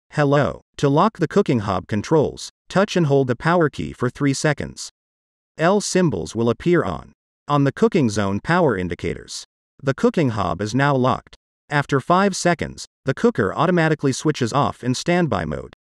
How to LOCK Gorenje Induction Hob Child LOCK
How to lock the Gorenje induction cooking hob controls, touch and hold the power key for 3 seconds. L symbols will appear on on the cooking zone power indicators. The cooking hob is now locked. After 5 seconds, the cooker automatically switches off in standby mode. Full video

I hope you found this video useful and you don't miss dinner.

🇷🇺 Hазвание видео: Как разблокировать индукционную плиту Gorenje - детский замок
🇩🇪 Video-Titel: Entriegelung eines Induktionskochfeldes Gorenje- Kindersicherung .